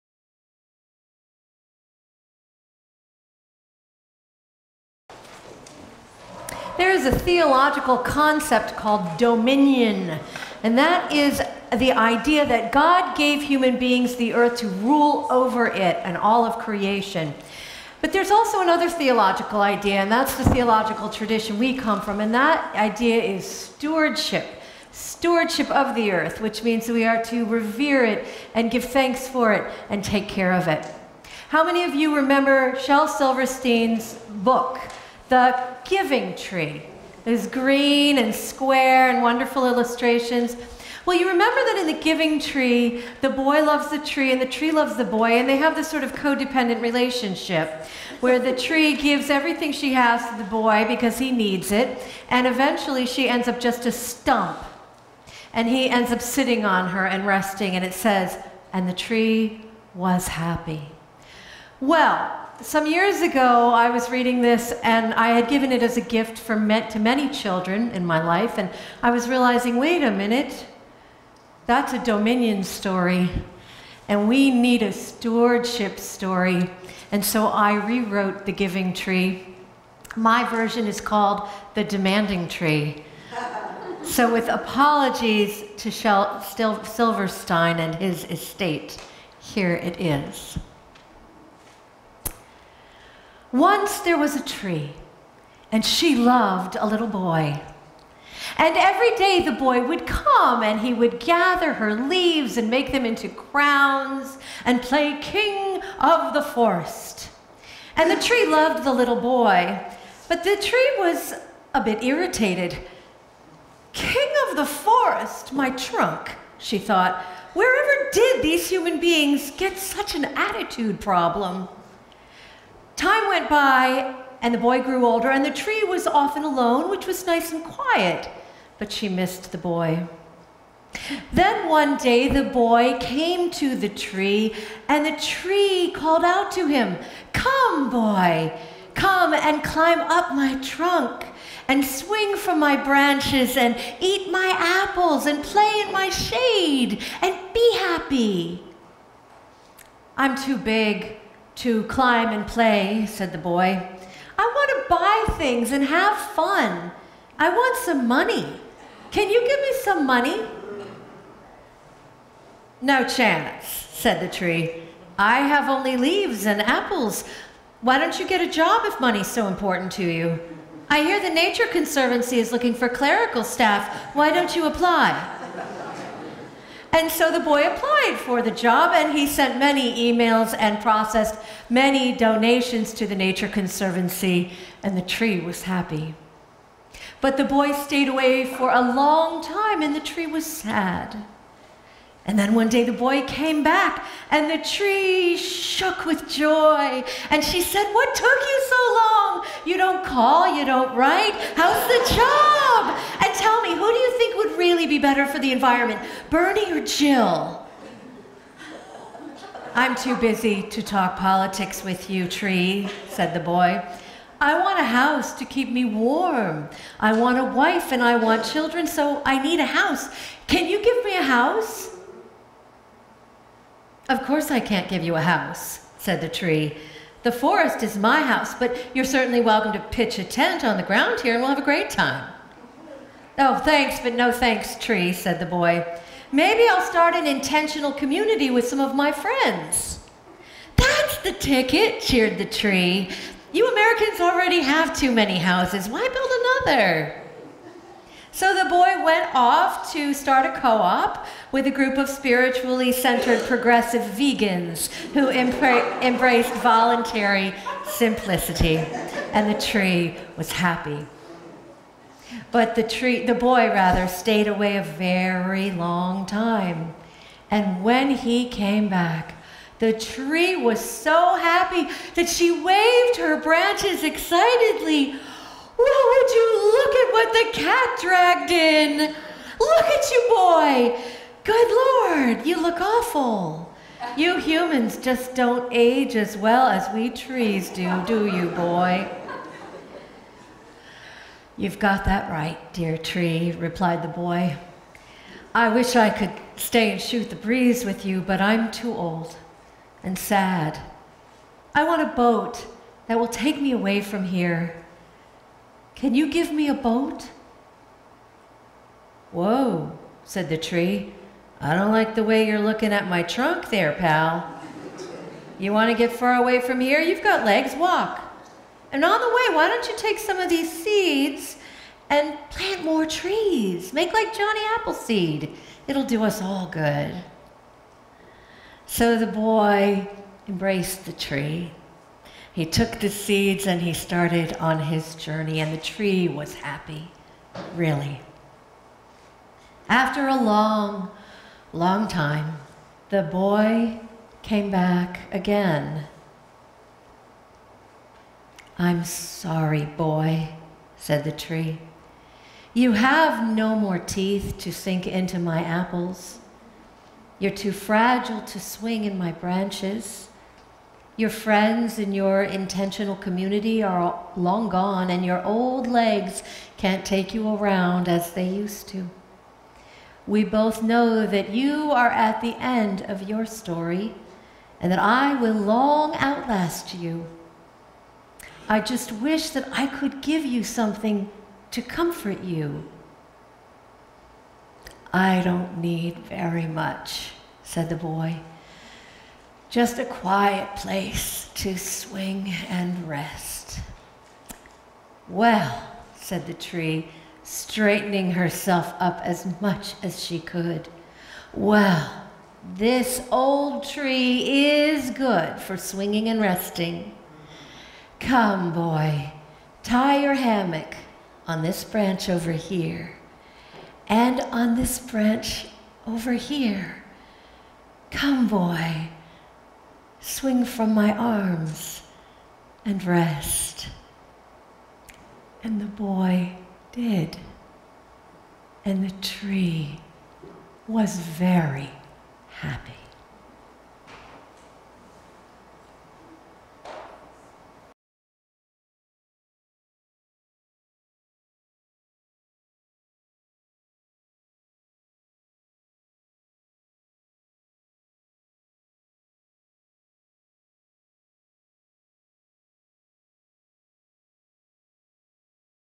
The Demanding Tree
Unitarian Universalist Church of Greater Lynn (MA)
Rev. Dr. Victoria Weinstein, Parish Minister
Sermon for May 1, 2016